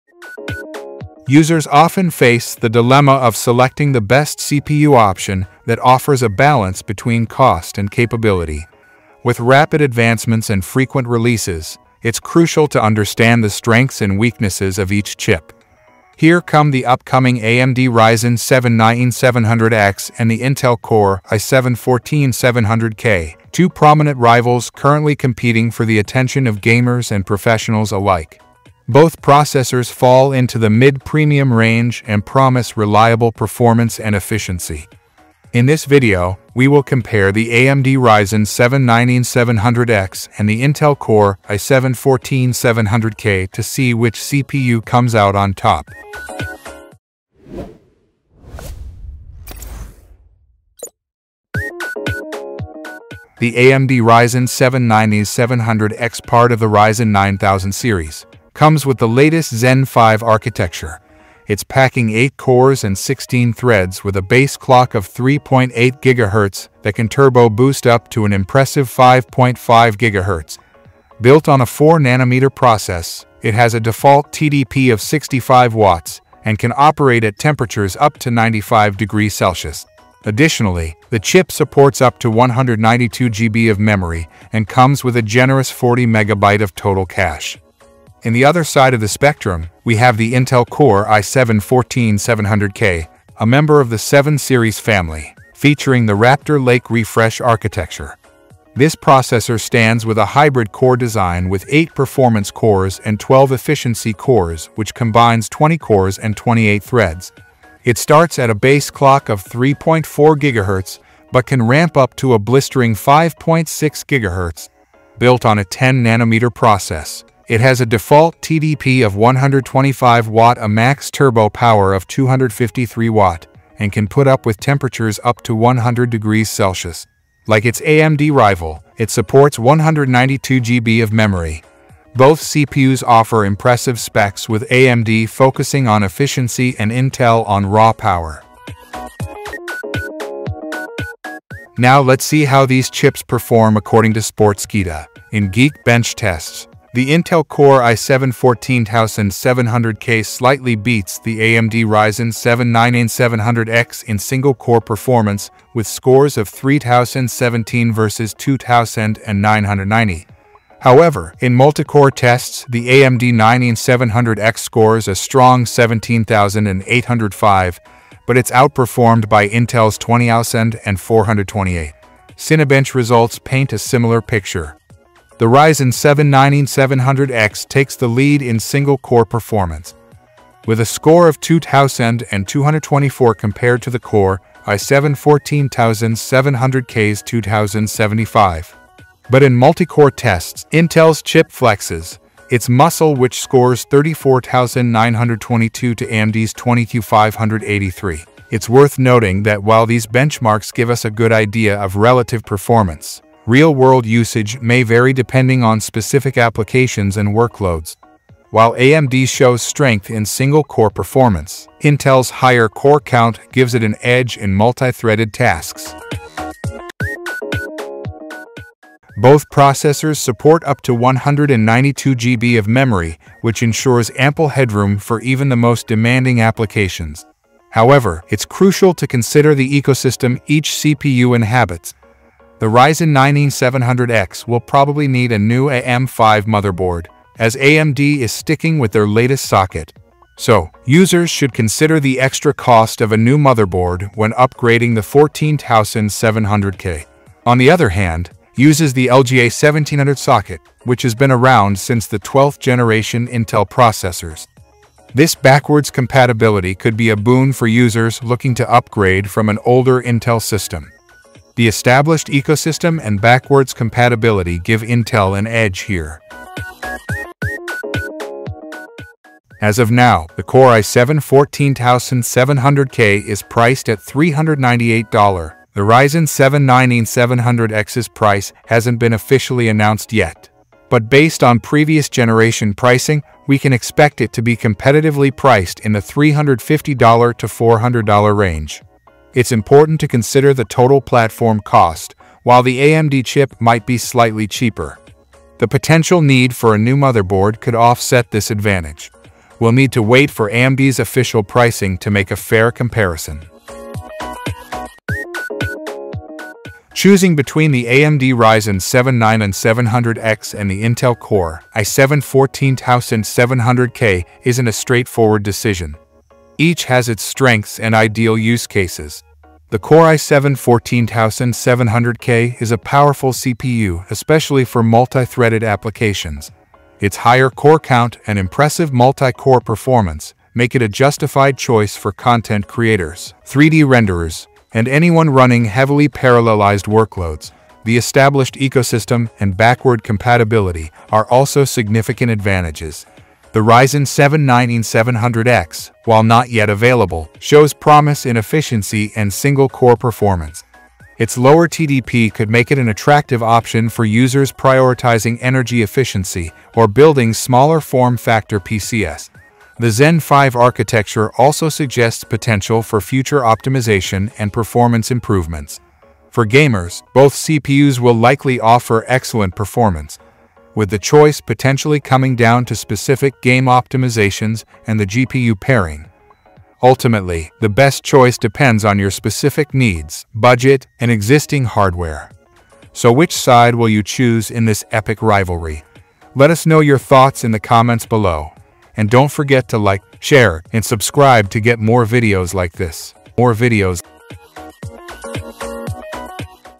Ryzen 7 9700X vs. Core i7 14700K: The Truth Exposed!
Which CPU—the AMD Ryzen 7 9700X or the Intel Core i7 14700K—should you pick in 2024? We have thoroughly compared all the important aspects, including gaming, multitasking, performance, and value. Whether you're a power user, gamer, or artist, this video will assist you in selecting the best CPU for your requirements.